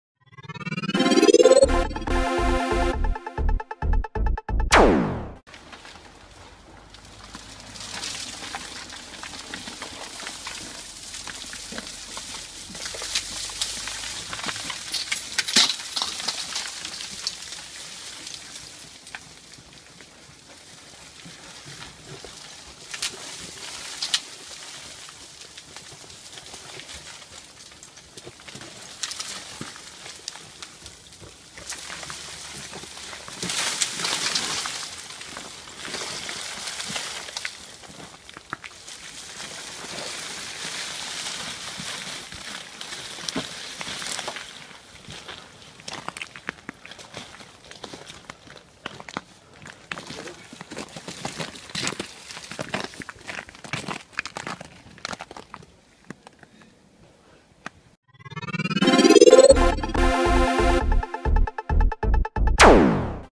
How to Break frozen branch from Tree without tools
Broken Tree branch Frozen after ice storm